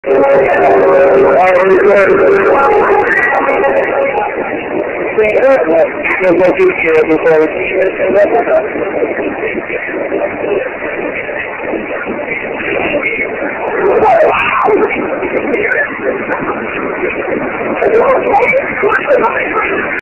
Double Fireball
My friend Natalya and some dude take shots of Everclear and spit them into a lighter, creating a double fireball.